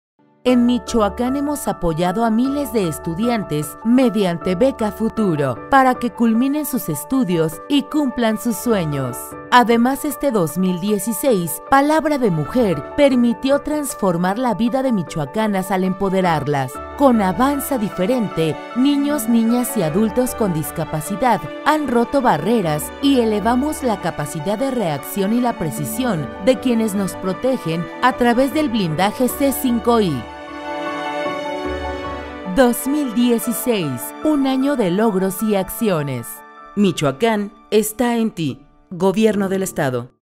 Año de logros - Beca Futuro - Gobierno de Michoacán
En Michoacán se han apoyado a miles de estudiantes mediante Beca Futuro.